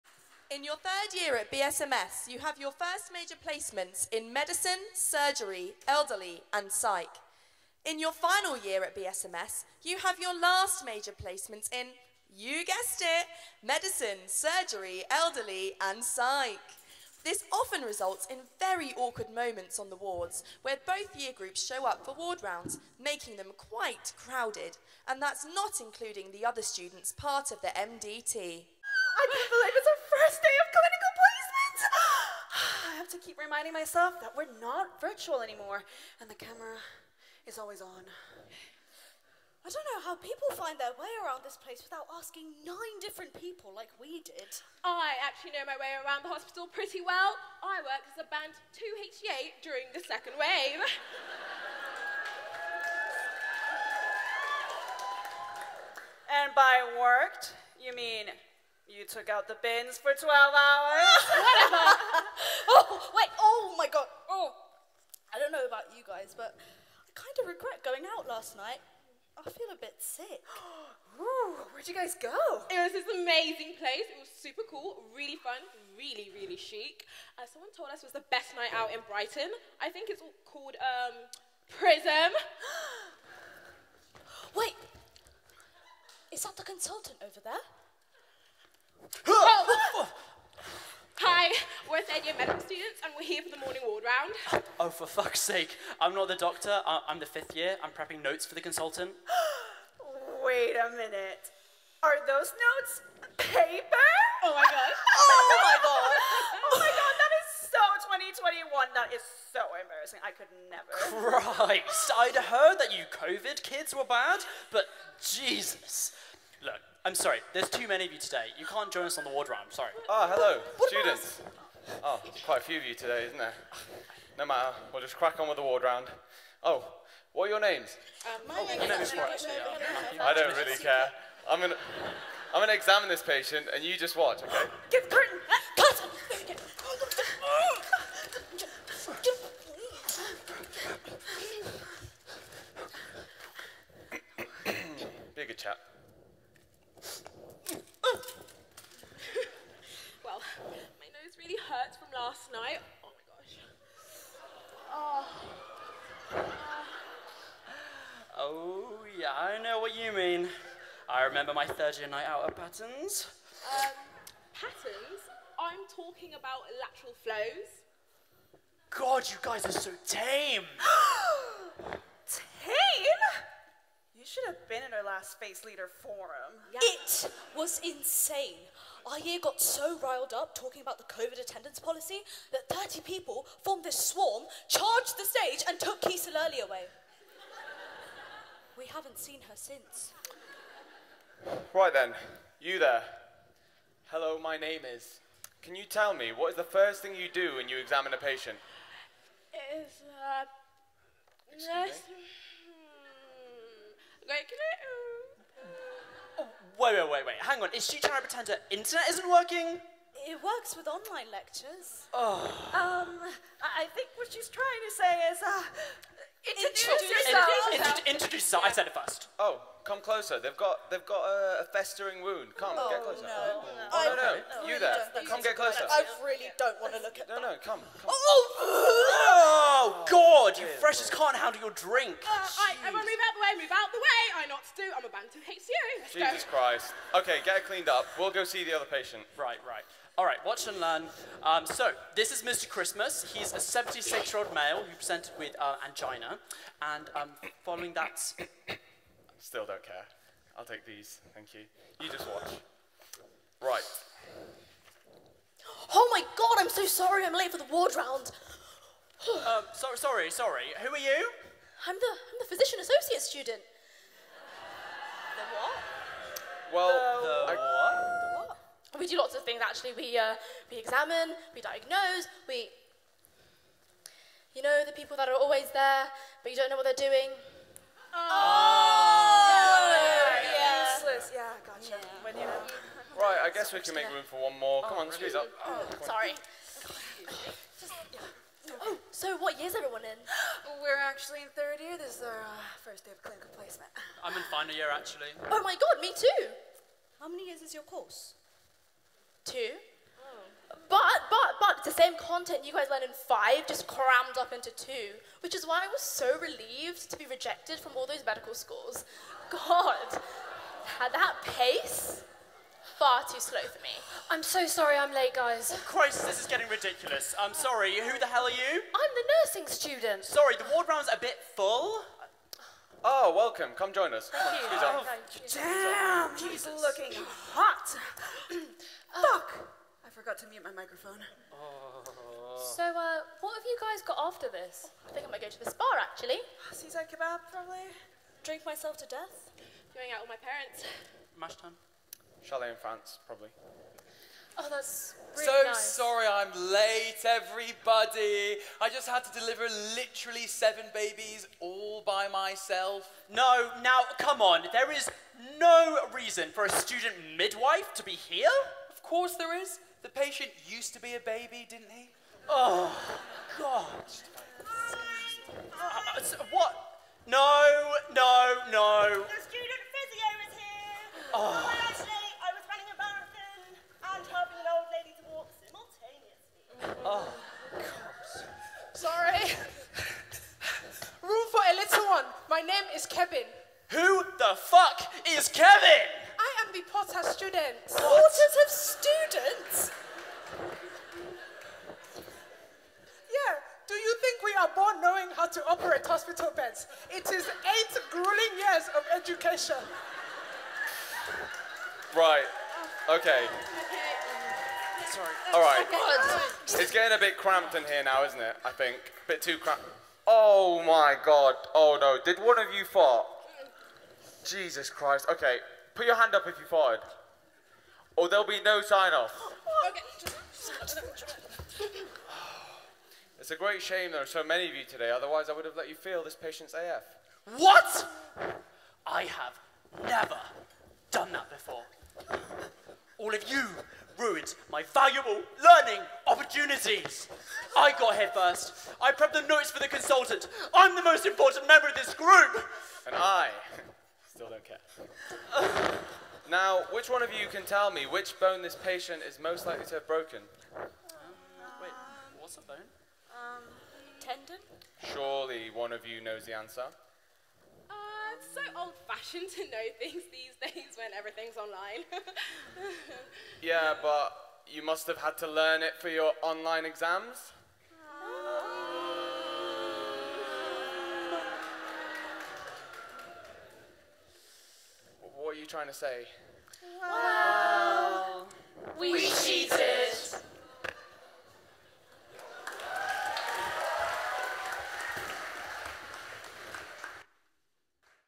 Student ward round | BSMS Medic Revue 2022
When you are at medical school, you go on ward rounds. Totally relatable.